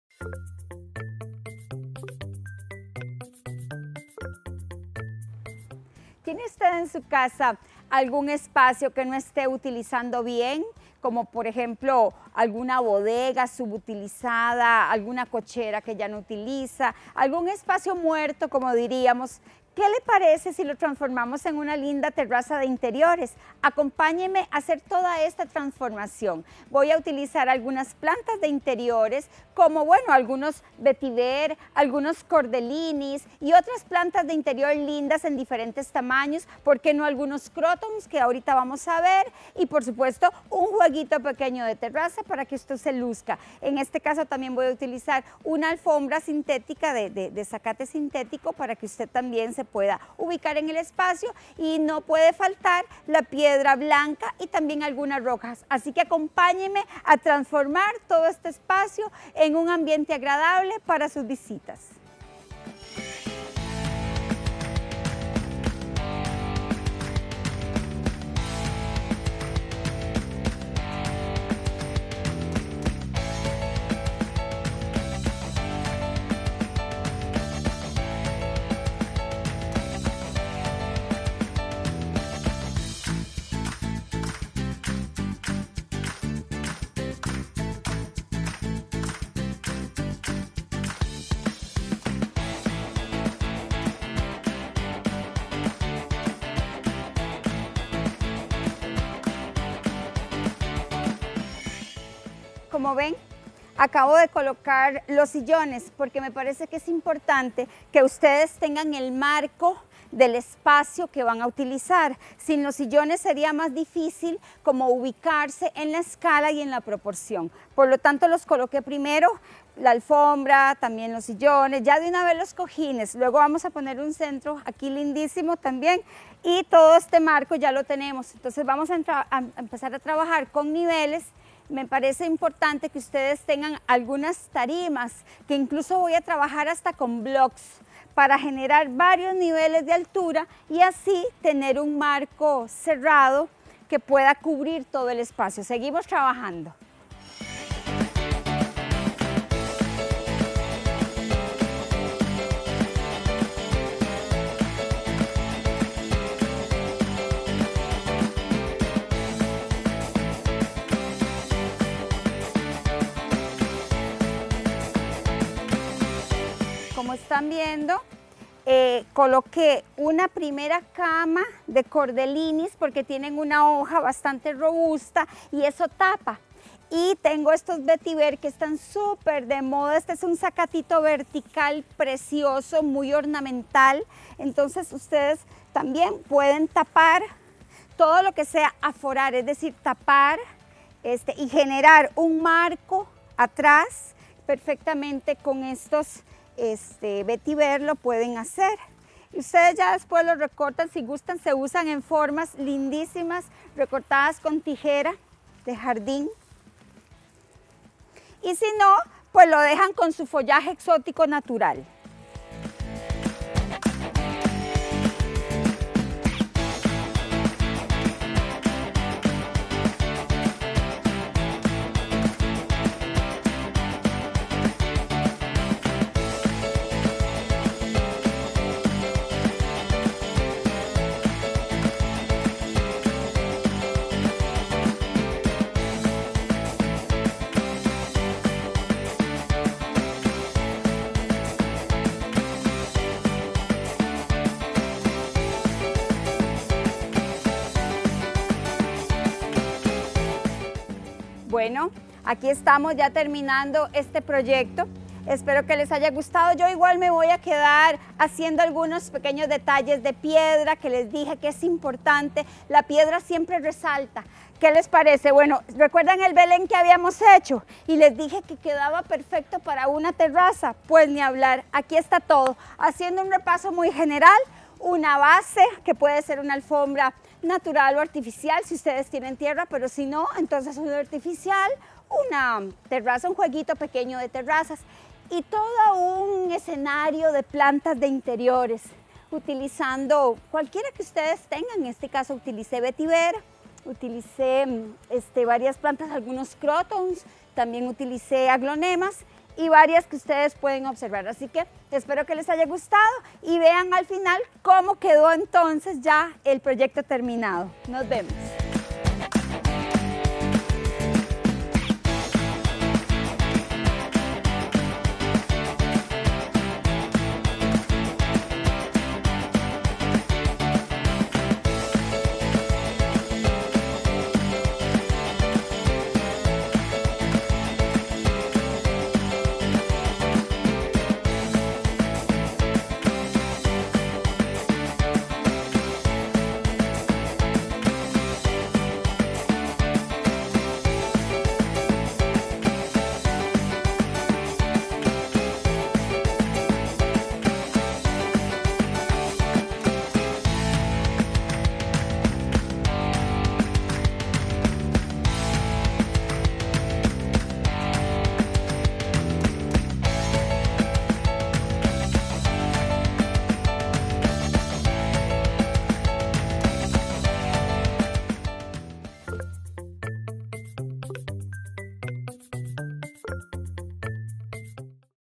Cómo hacer una Terraza.
En este segmento aprenderemos a hacer una terraza en esos espacios vacíos de nuestro hogar.